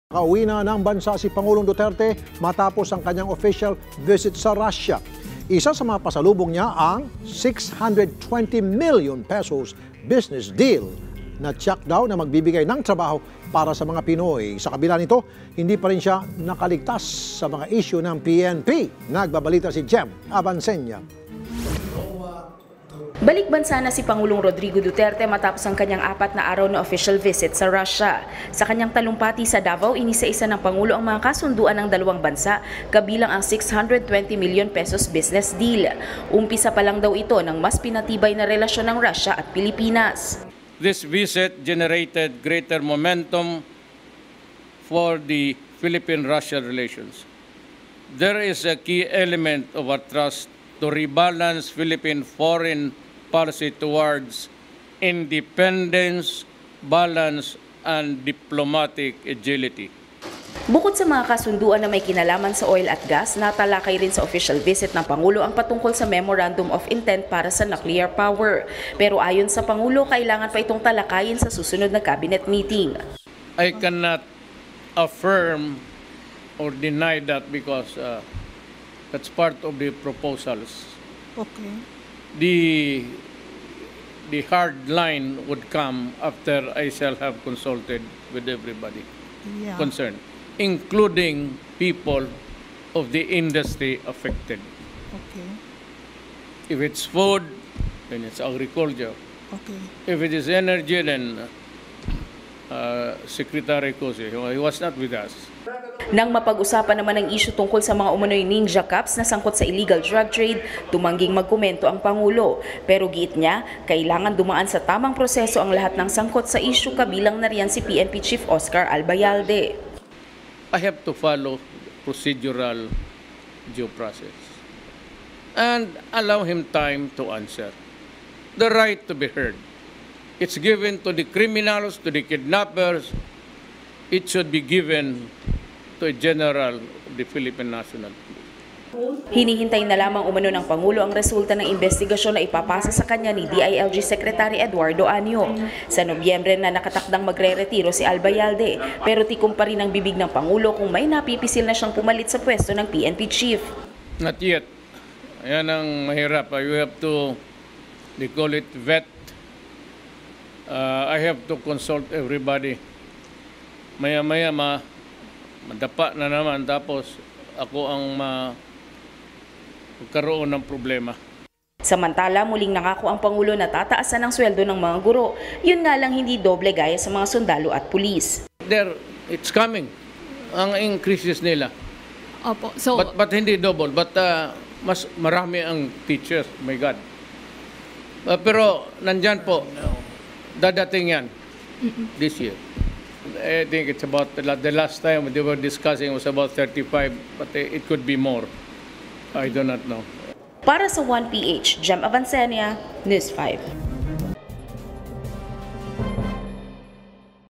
620M Peso Business Uwi ni Pres. DU30 Mula Russia | ONE BALITA
Ibinida ni Pangulong Duterte ang 620 million pesos business deal napagkasunduan sa pagitan ng bansang PIlipinas at Russia mula sa kanyang official visit sa Russia.

President Duterte announces 620 million pesos business deal agreed between Philippines and Russia from his official visit to Russia.